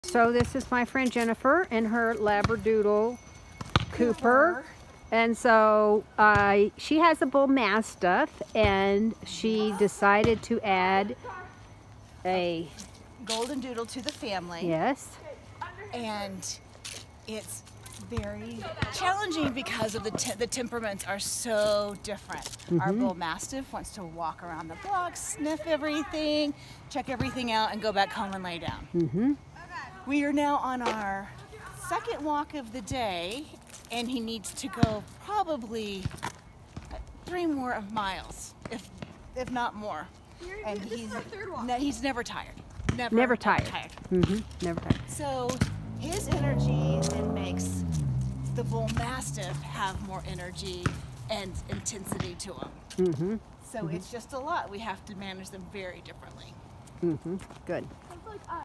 Labradoodle Vs Bullmastiff LonelyCreek Bullmastiff
In A park near their home throwing the ball for Cooper their Labradoodle